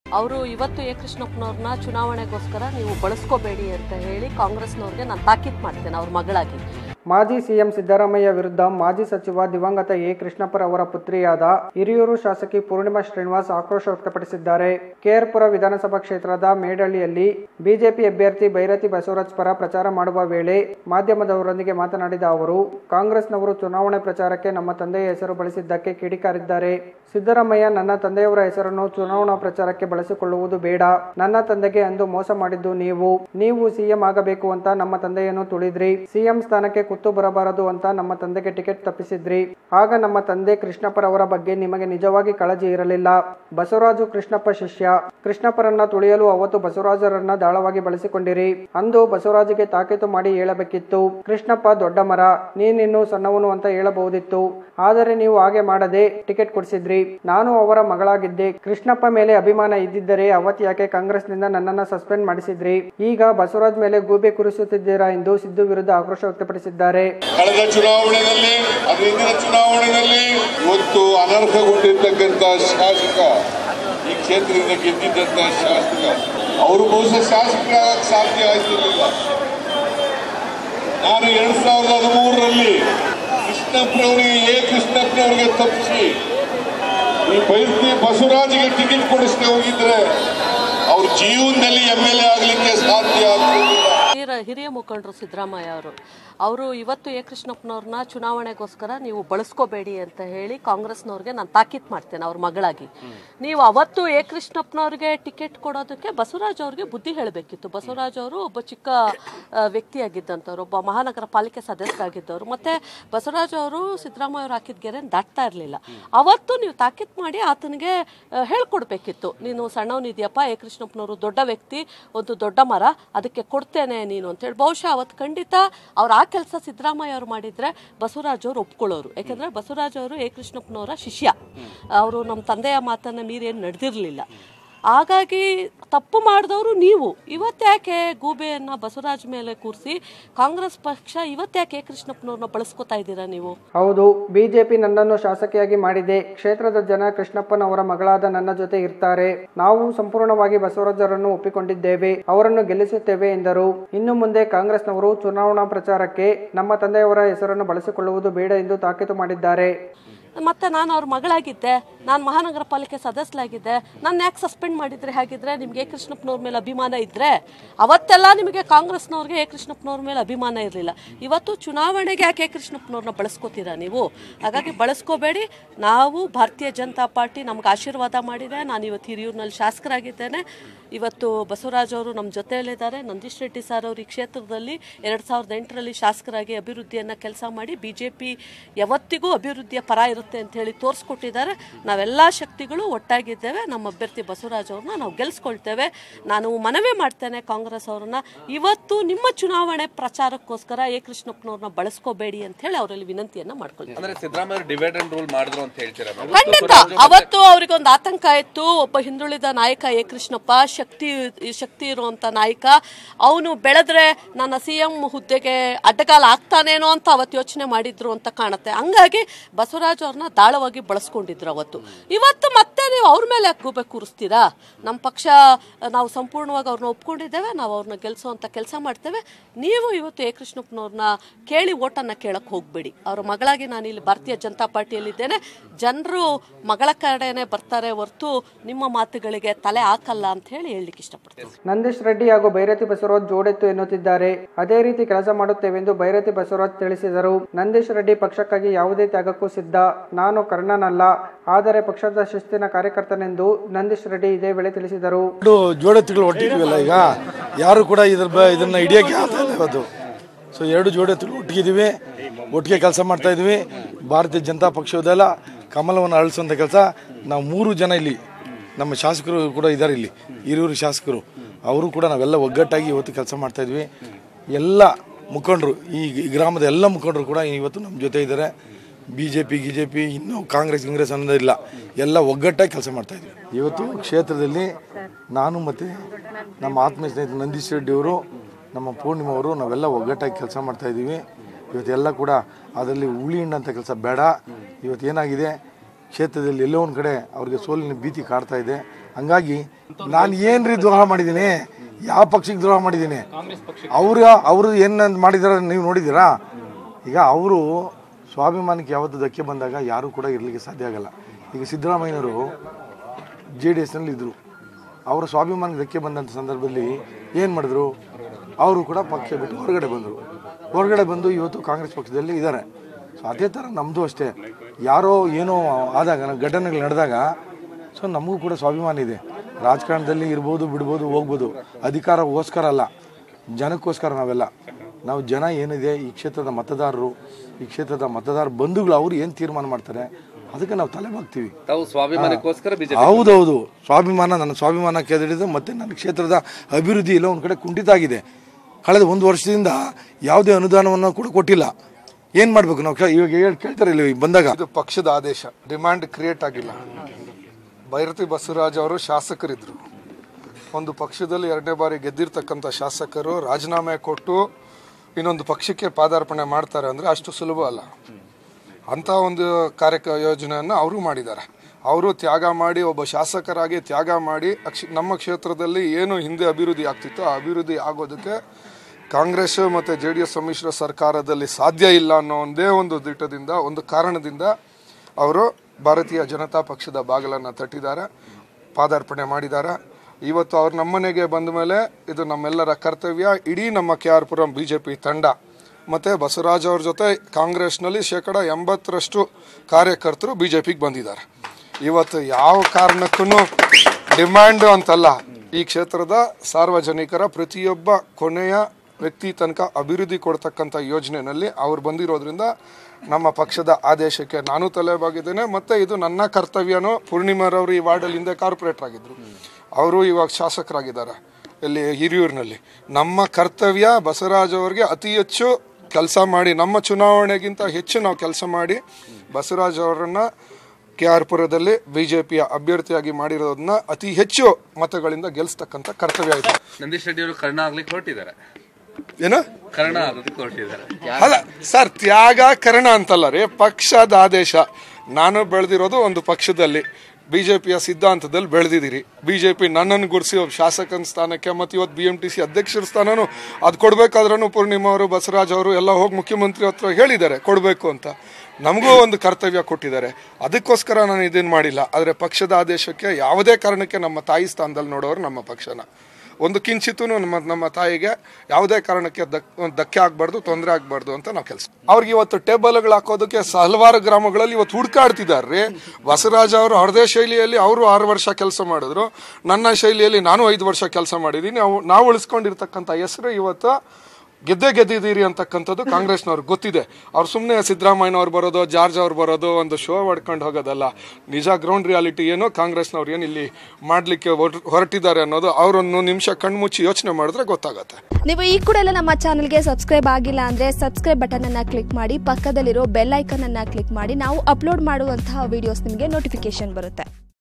ಚುನಾವಣೆ ಪ್ರಚಾರಕ್ಕೆ ತಮ್ಮ ತಂದೆಯ ಹೆಸರು ಬಳಸಿದ್ದಕ್ಕೆ ಶಾಸಕಿ ಪೂರ್ಣಿಮಾ ಕಿಡಿ ಕಿಡಿ | BLOOMTV NEWS
ಚುನಾವಣೆ ಪ್ರಚಾರಕ್ಕೆ ತಮ್ಮ ತಂದೆಯ ಹೆಸರು ಬಳಸಿದ್ದಕ್ಕೆ ಶಾಸಕಿ ಪೂರ್ಣಿಮಾ ಕಿಡಿ ಕಿಡಿ.

ಕೆ.ಆರ್.ಪುರಂ ಪ್ರಚಾರದ ವೇಳೆ ಸಿದ್ದು ವಿರುದ್ದ ವಾಗ್ದಾಳಿ.

ಸಿದ್ದರಾಮಯ್ಯ ನನ್ನ ತಂದೆಯವರ ಹೆಸರನ್ನು ಚುನಾವಣಾ ಪ್ರಚಾರಕ್ಕೆ ಬಳಸಿಕೊಳ್ಳುವುದು ಬೇಡ.

ನನ್ನ ತಂದೆಗೆ ಅಂದು ಮೋಸ ಮಾಡಿದ್ದು ನೀವು.

ನೀವು ಸಿಎಂ ಆಗಬೇಕು ಅಂತ ನಮ್ಮ ತಂದೆಯನ್ನು ತುಳಿದ್ರಿ.

ಸಿಎಂ ಸ್ಥಾನಕ್ಕೆ ಕುತ್ತು ಬರಬಾರದು ಅಂತ ನಮ್ಮ ತಂದೆಗೆ ಟಿಕೇಟ್ ತಪ್ಪಿಸಿದಿರಿ.

ನಮ್ಮ ತಂದೆ ಕೃಷ್ಣಪ್ಪ ಬಗ್ಗೆ ನಿಮಗೆ ನಿಜವಾಗಿ ಕಾಳಜಿ ಇರಲಿಲ್ಲ.

ಇದ್ದಿದ್ದರೆ ಅವತ್ತೇ ಬಸವರಾಜ್ ಗೆ ಬುದ್ದಿ ಹೇಳಬಹುದಿತ್ತು.

ಬಸವರಾಜ್ ಅವತ್ತು ನೀವು ಹಾಕಿದ ಗೆರೆ ದಾಟುತ್ತಿರಲಿಲ್ಲ.

ಬಸವಾರಜು ಕೃಷ್ಣಪ್ಪ ಶಿಷ್ಯ, ಕೃಷ್ಣಪ್ಪರನ್ನ ತುಳಿಯಲು ಅವತ್ತು ಬಸವರಾಜ್ ರನ್ನ ದಾಳವಾಗಿ ಬಳಸಿಕೊಂಡಿರಿ.

ಬಸವರಾಜ್ ಗೆ ತಾಕೀತು ಮಾಡಿ ಹೇಳಬೇಕಿತ್ತು,

ಬಿಜೆಪಿ ನನ್ನನ್ನು ಶಾಸಕಿಯಾಗಿ ಮಾಡಿದೆ, ಕ್ಷೇತ್ರದ ಜನ ಕೃಷ್ಣಪ್ಪನವರ ಮಗಳಾದ ನನ್ನ ಜೊತೆ ಇರ್ತಾರೆ.

ನಾವು ಸಂಪೂರ್ಣವಾಗಿ ಬಸವರಾಜ್ ರನ್ನು ಒಪ್ಪಿಕೊಂಡಿದ್ದೇವೆ.

ಕಾಂಗ್ರೆಸ್ ನವರು ಚುನಾವಣೆ ಪ್ರಚಾರಕ್ಕೆ ನಮ್ಮ ತಂದೆ ಯನ್ನು ಬಳಸಿಕೊಳ್ಳುವುದು ಬೇಡ.

ಸಿದ್ದರಾಮಯ್ಯ ವಿರುದ್ದ ಪೂರ್ಣಿಮಾ ಆಕ್ರೋಶ.

ನಂದೀಶ್ ರೆಡ್ಡಿ ಹಾಗೂ ಬೈರತಿಬಸವರಾಜು ಜೊಡೇತ್ತು

ಅದೇ ರೀತಿ ಕೆಲಸ ಮಾಡುತ್ತೇವೆಂದು ಬೈರತಿ ಬಸವರಾಜು ತಿಳಿಸಿದರು.

ನಂದೀಶ್ ರೆಡ್ಡಿ ಪಕ್ಷಕ್ಕಾಗಿ ಯಾವುದೇ ತ್ಯಾಗಕ್ಕೂ ಸಿದ್ದ

ನಾನು ಕರ್ಣನಲ್ಲ ಆದರೆ ಪಕ್ಷದ ಶಿಸ್ತಿನ ಕಾರ್ಯಕರ್ತನೆಂದ ನಂದೀಶ್ ರೆಡ್ಡಿ ಹೇಳಿಕೆ ನೀಡಿದರು